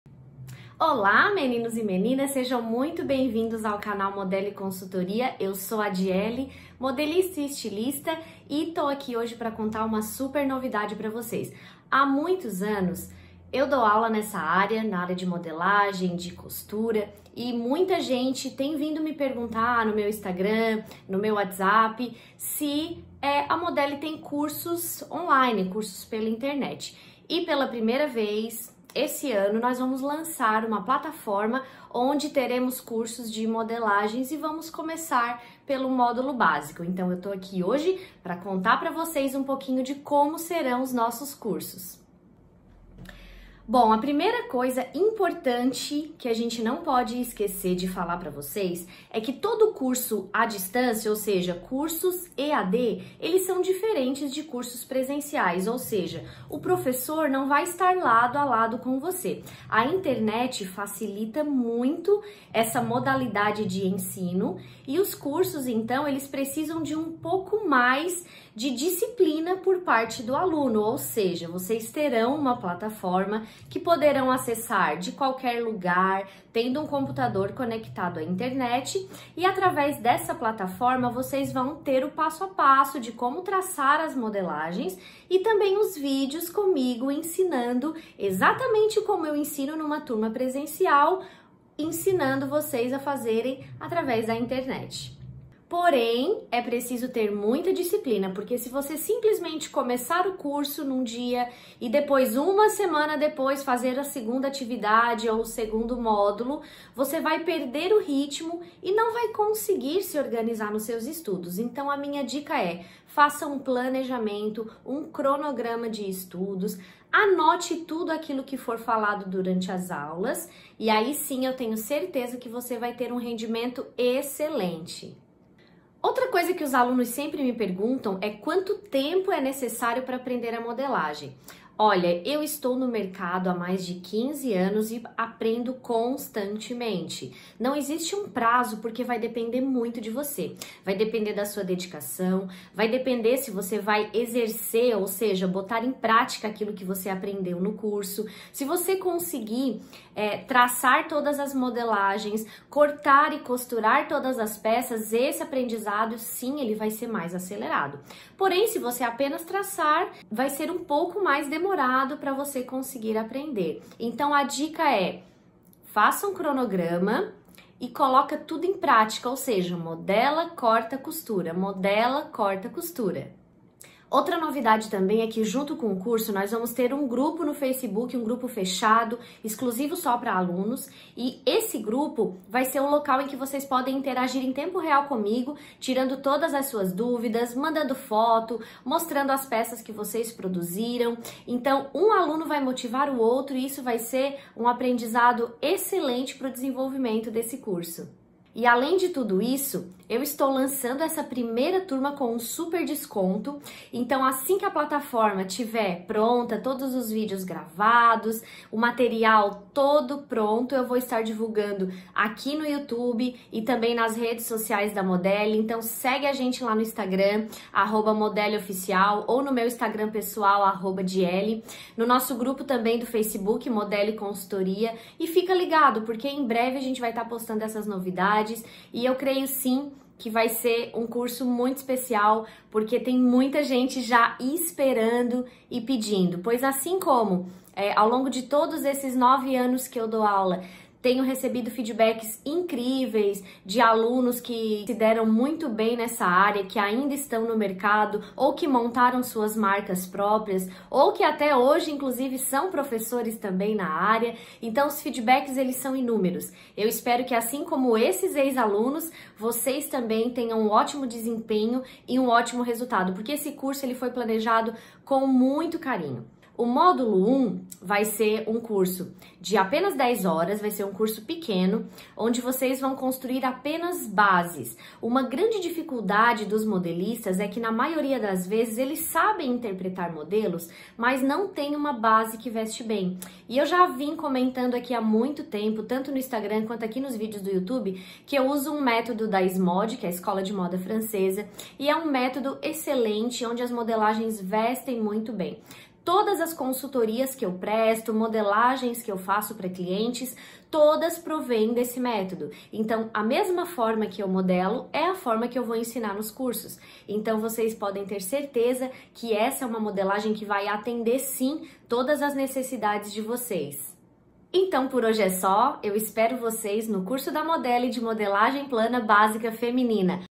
Cursos de modelagem à distância funcionam?
Já vou logo respondendo a  pergunta do vídeo...DEPENDE!
Olá meninos e meninas do mundo da modelagem! Depois de tanta gente me perguntar no meu insta pessoal @dyeli ou no da modele @modeleoficial se nós tinhamos cursos de modelagem online, finalmente eu decidi arregassar as mangas e começar a produzir um curso totalmente voltado para a prática de modelagem e 2020 foi o ano escolhido para o lançamento desse novo projeto!
Nesse vídeo eu explico quais os desafios do EAD (ensino à distância) e o que o aluno precisa para conseguir o resultado esperado, falo também do tempo necessário para o aprendizado e da necessidade de pilotar todas as peças (ou seja modela, corta e costura).
Para os alunos da primeira turma teremos um grupo fechado no facebook exclusivo somente para alunos onde vocês poderão contar com a minha mentoria e também será um espaço para integração e motivações entre colegas.
Bora aprender modelagem comigo?